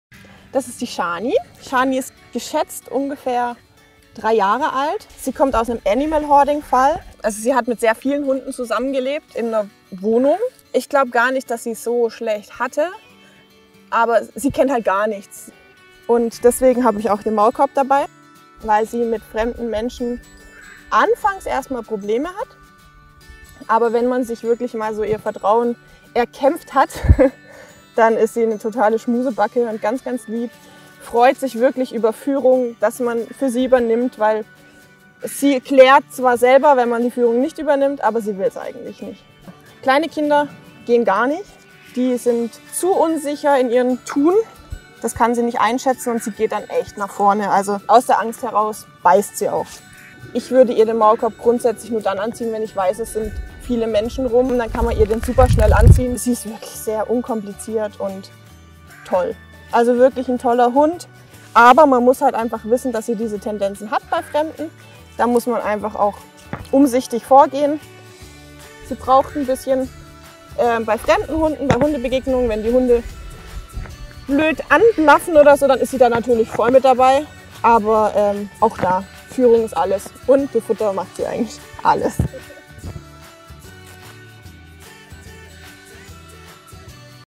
Langzeitinsassin Shani
Hier stellen wir unsere Langzeitinsassin Shani vor, in der Hoffnung auf baldige Vermittlung.

Ein großes Dankeschön an Natascha Schindler und Yvonne Westerbeck für das Filmen und schneiden...